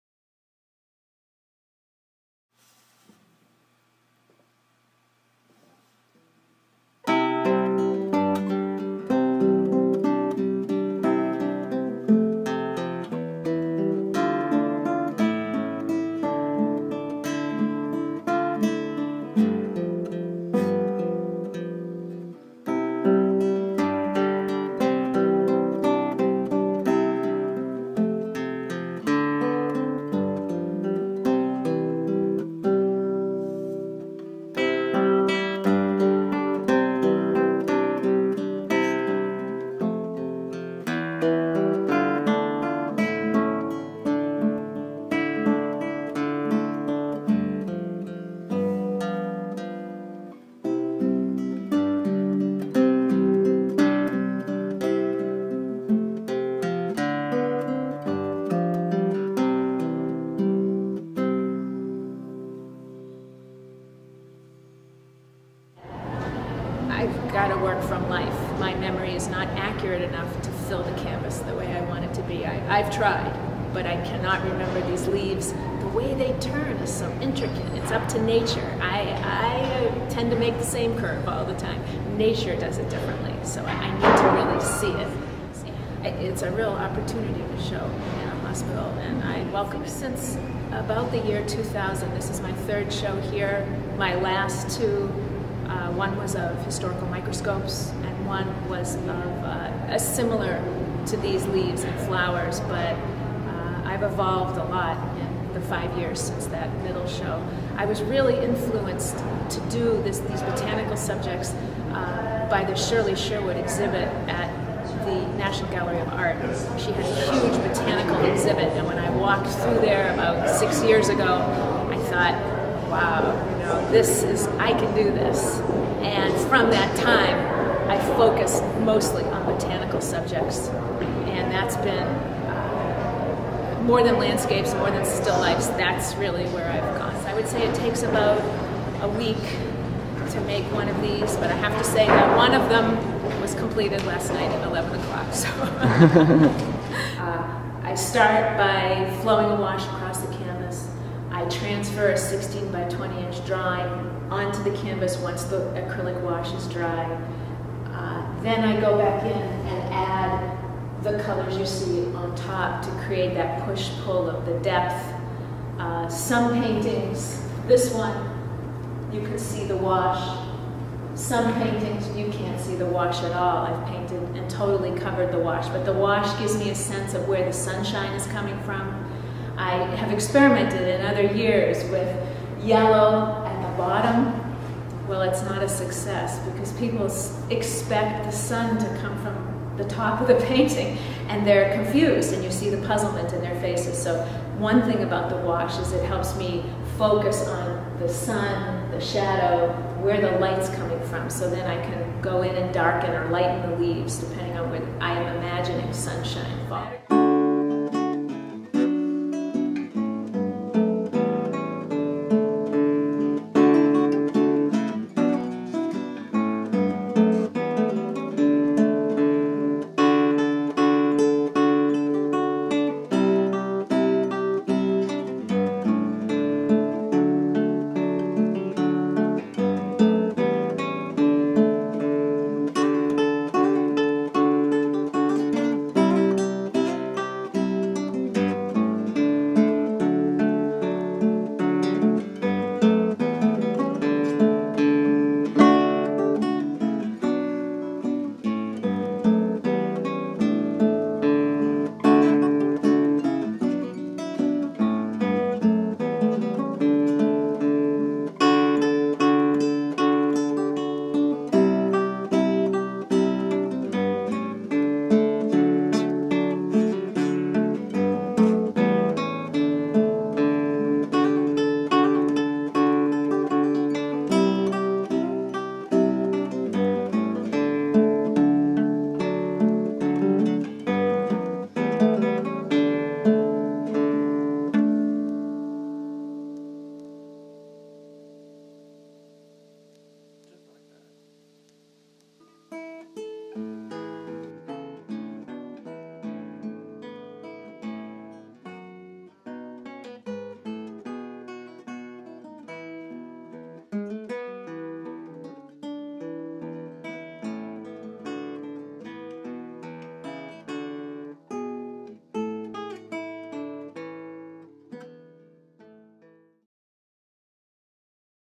⫷ The Art of Sue Fierston ⫸
Painter and feltmaker Sue Fierston Exhibition 2012 "Leaves Three Ways" at the Lombardi Cancer Center, Washington, DC.